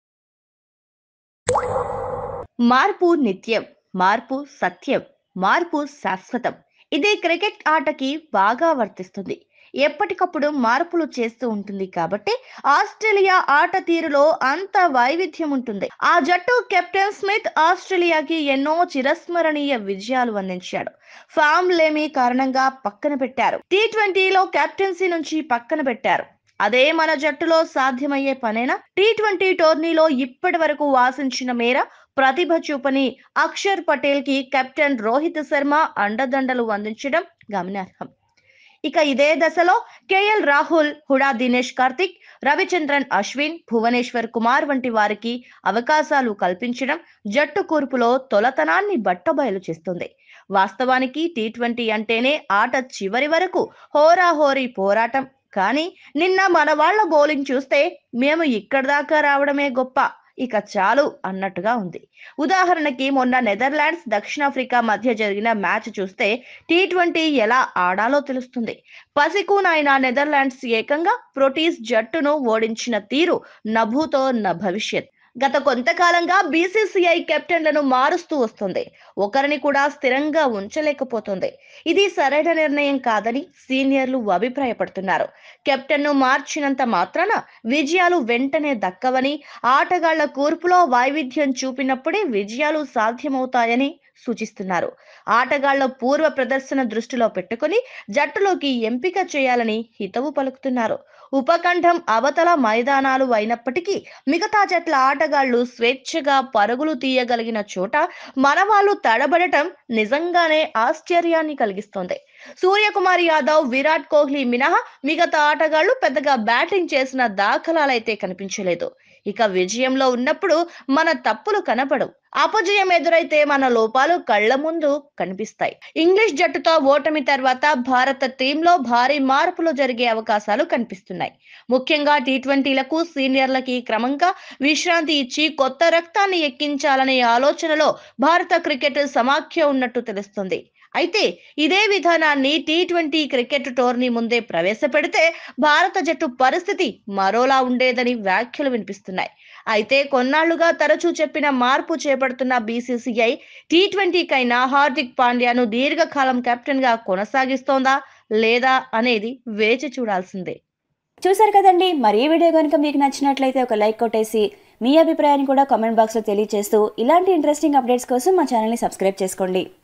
Hardik Pandya will definitely take over as T20I captain || Ok Telugu
Watch Hardik Pandya will definitely take over as T20I captain video in Ok Telugu